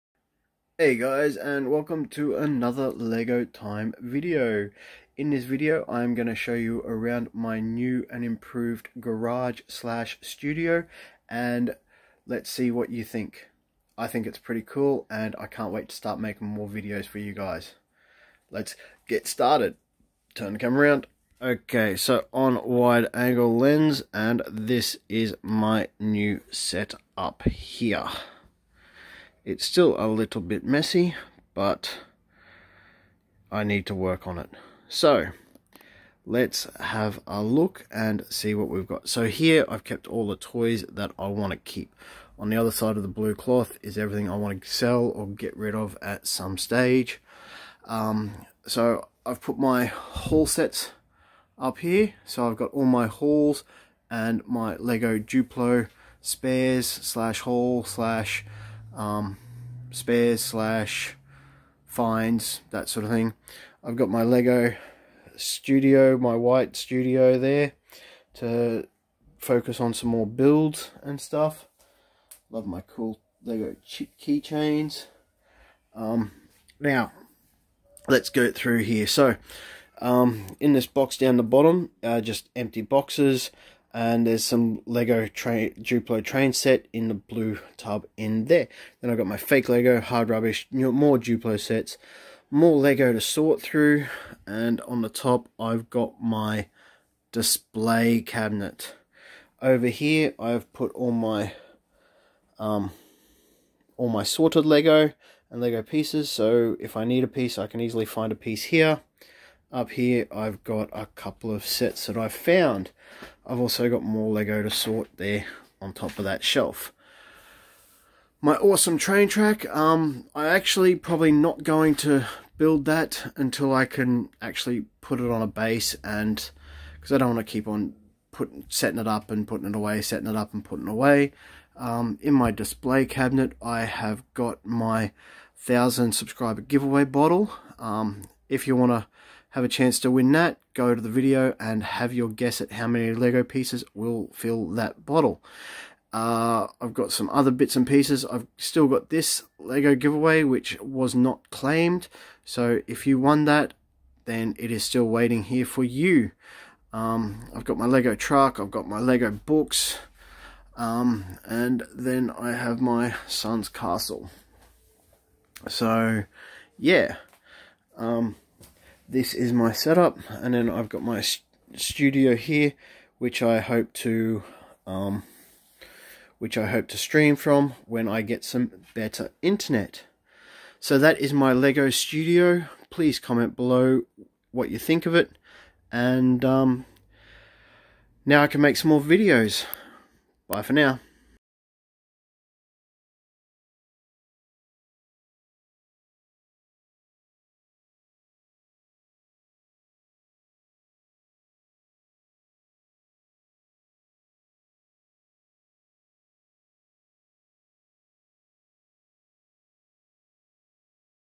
Cleaning Up My Studio - Part 2
I finally get my studio clean enough to start making more Lego videos.

*Some Lego sets featured on this channel will be for sale